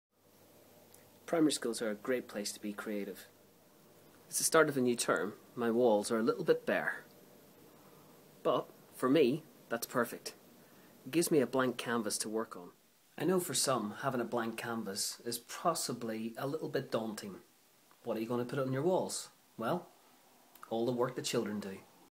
Tip 10 Leave a display blank
Leaving one display blank isn't a sign of laziness but providing you and your class a space which can be used when required. A working wall, quick displays of daily work, it can constantly change but can always be blanked quickly.